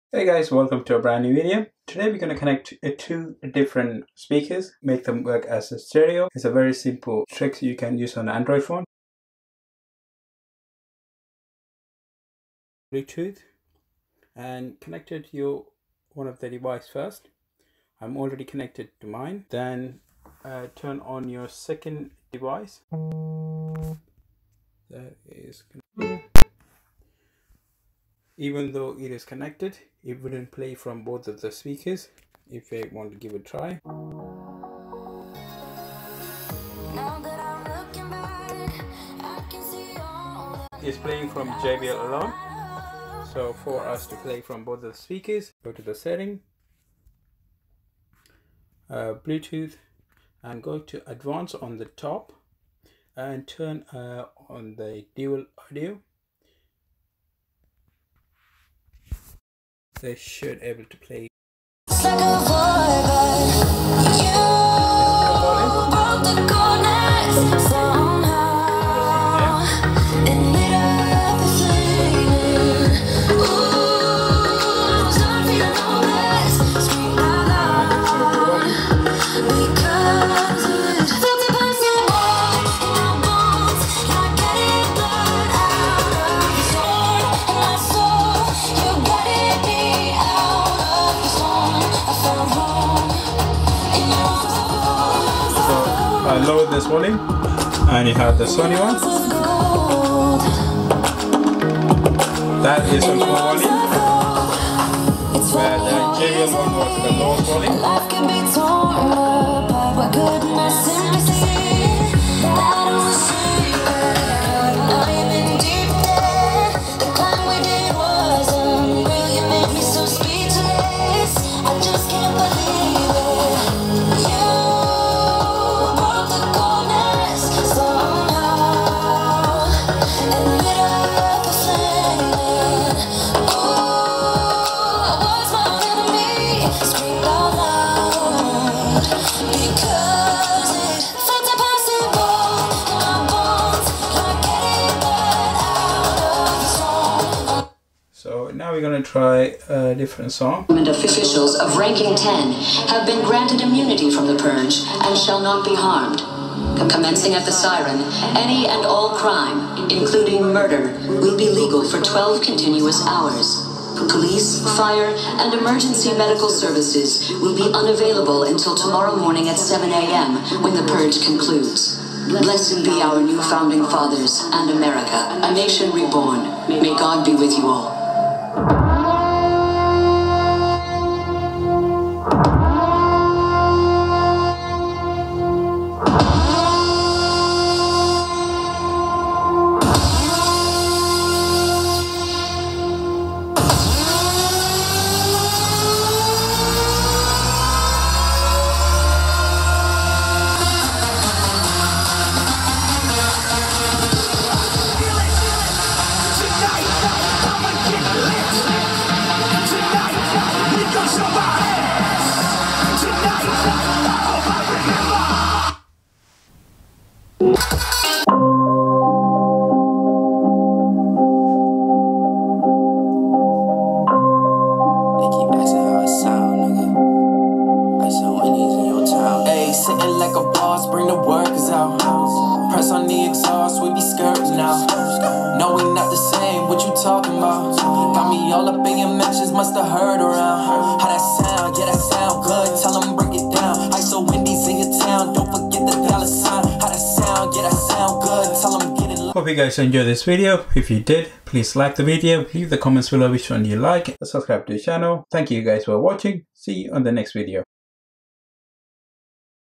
How to Connect Multiple Bluetooth Speakers to Android Device
Easy way  to connect Two bluetooth speakers with phone. Enjoy the dual audio.
JBL Flip 5
Sony XB33